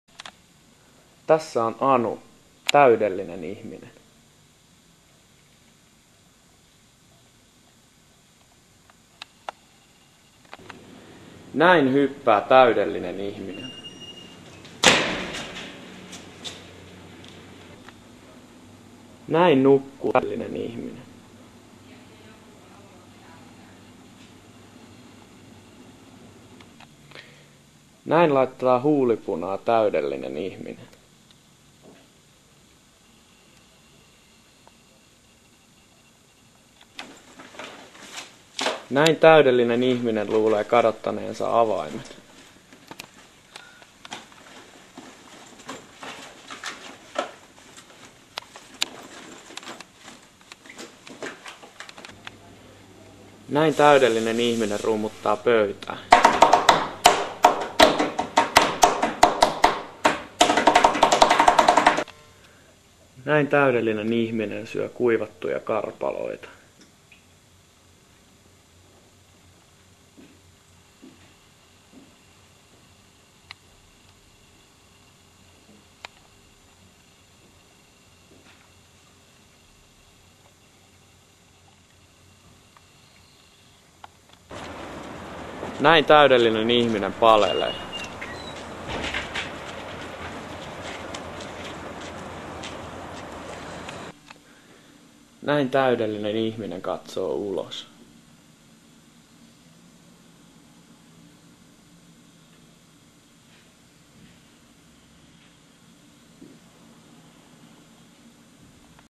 Täydellinen ihminen: Anu
Opettajien kulttuurikurssi: Kauneus - media - minäkuva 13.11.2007.
Videotyöpaja "Täydellinen ihminen" Jørgen Lethin lyhytelokuvan hengessä.
Työpajan ohjaaja Ismo Kiesiläinen